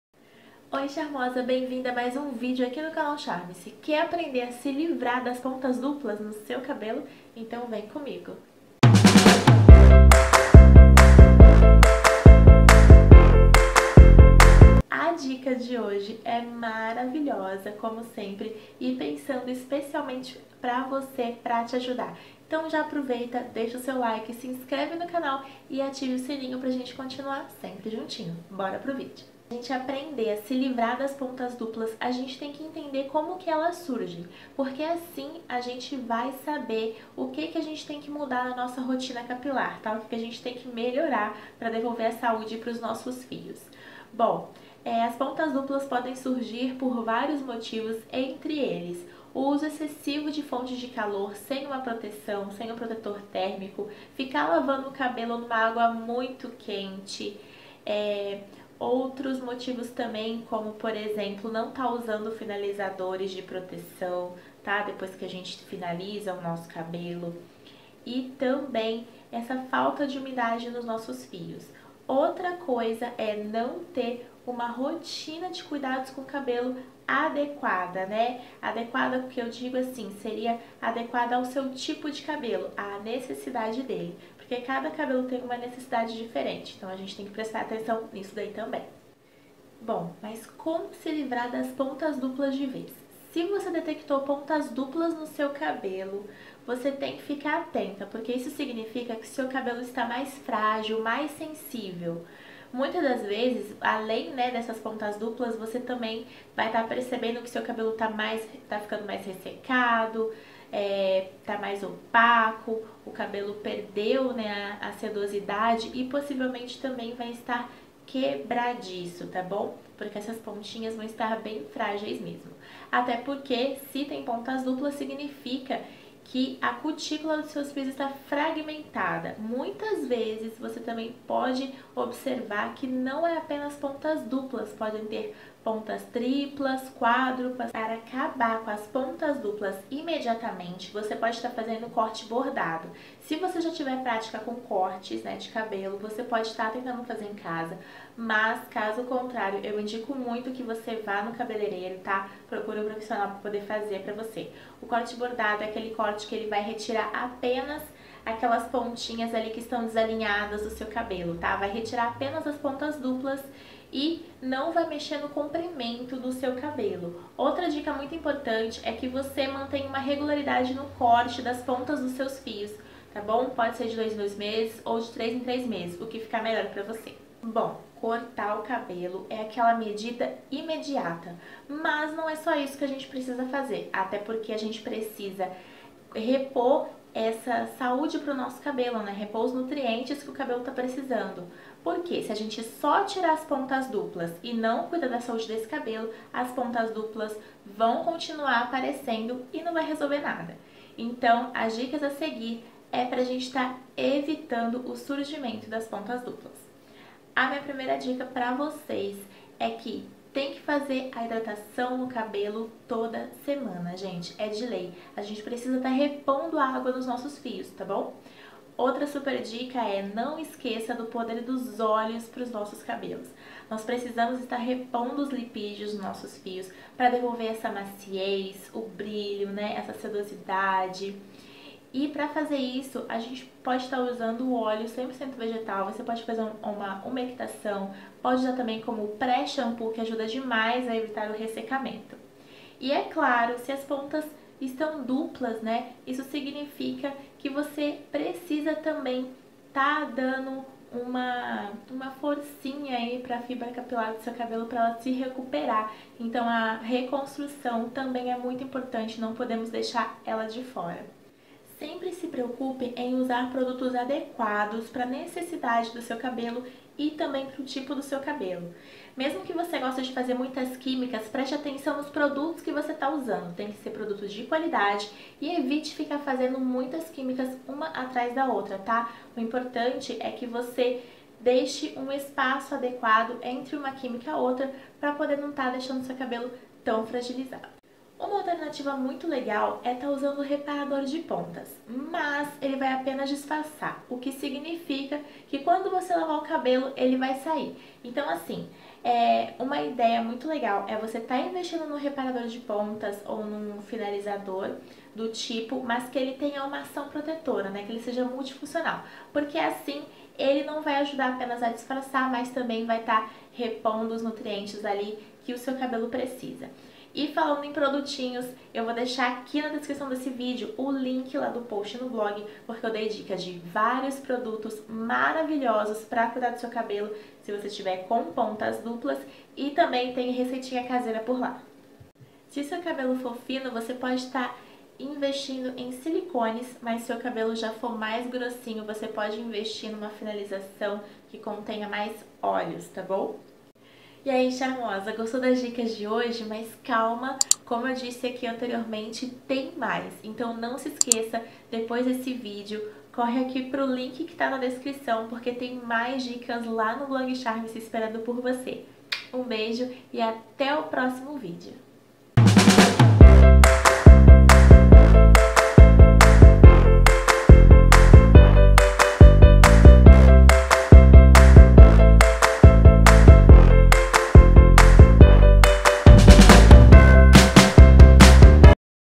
COMO ACABAR com as PONTAS DUPLAS do cabelo l Charme-se
Veja como acabar com as PONTAS DUPLAS DO CABELO. Revelei algumas das minhas dicas para manter os fios sempre saudáveis.